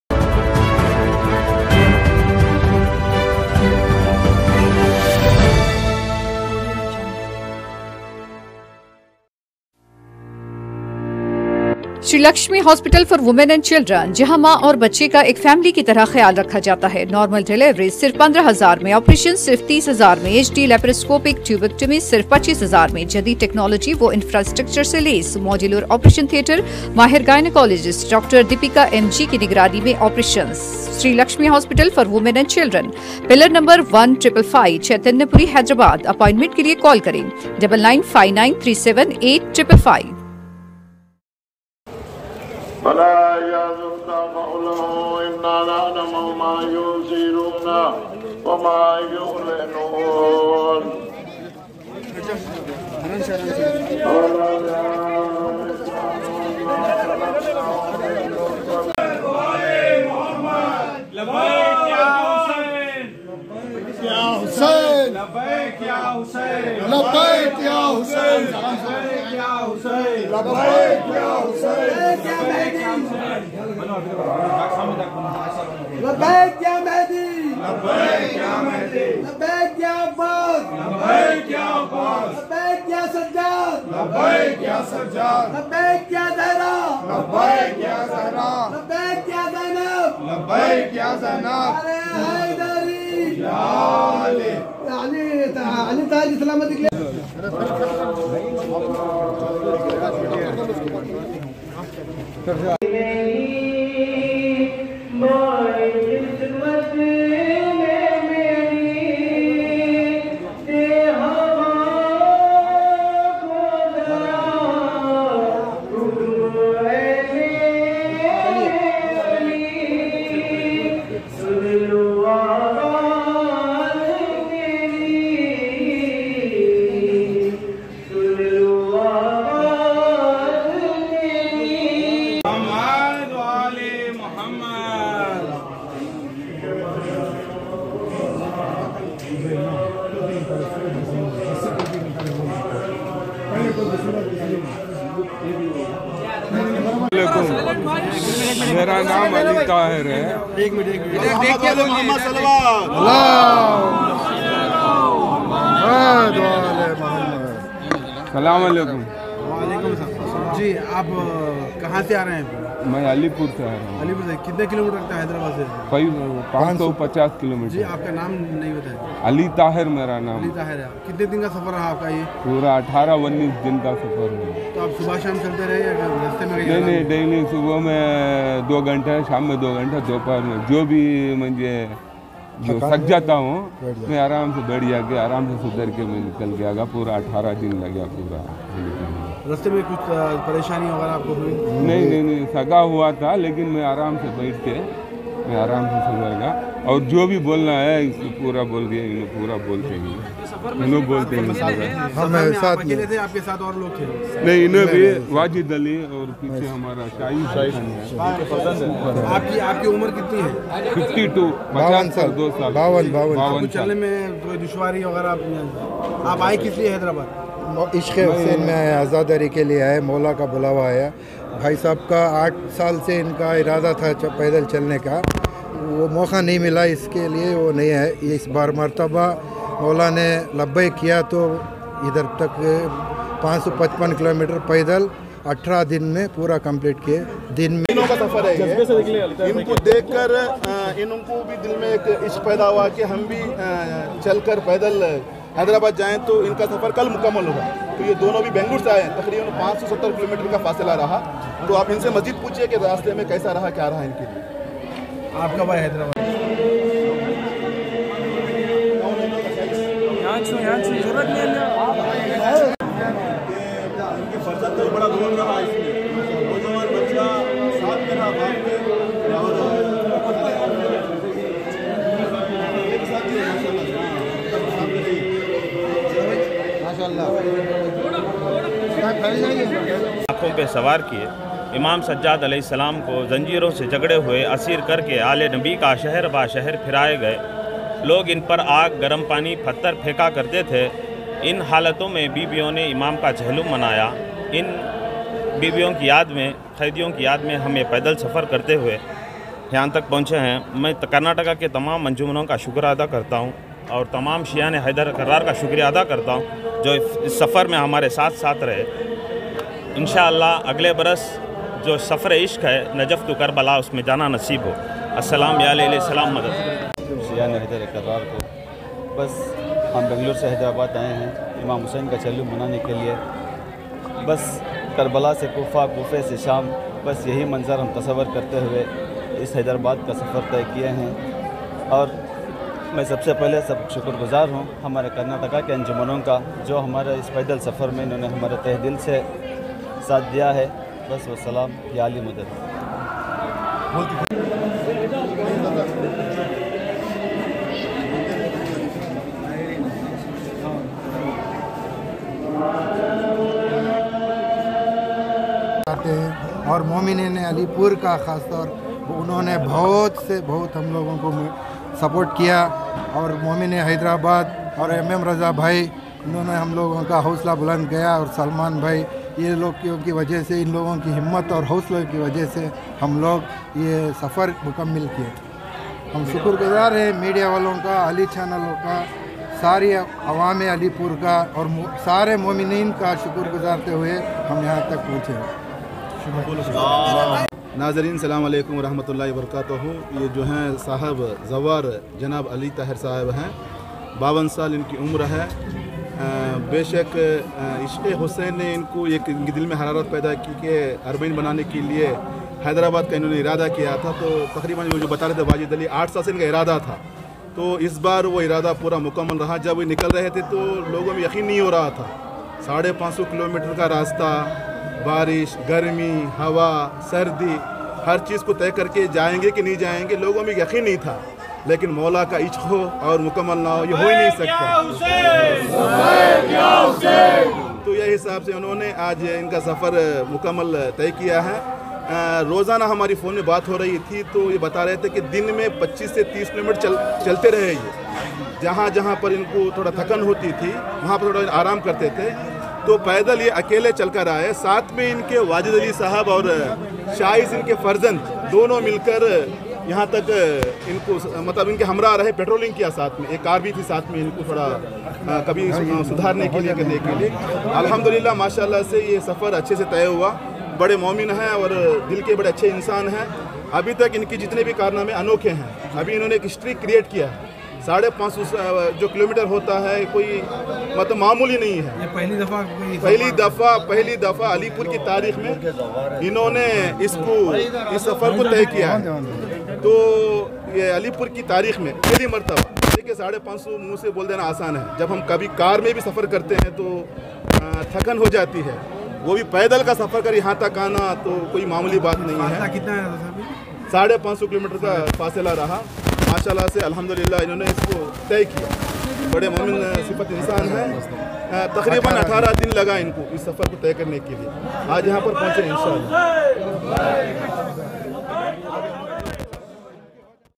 Hyderabad....Safar e Ishaq 55 Saala Buzurg ka Alipur Karnataka se Hyderabad Arbeaan ke liye paidal
Safar Paidal Zawar Ali Taher joh Alipur Karnataka se Hyderabad Arbayeen e Imam Hussain A.S. karne ke liye 570 Kilometers ka Fasla Paidal yaye karte huwa Aaye hai inke Humraah unke Farzand aur Mir Wajid Ali Bhi Maujood teh.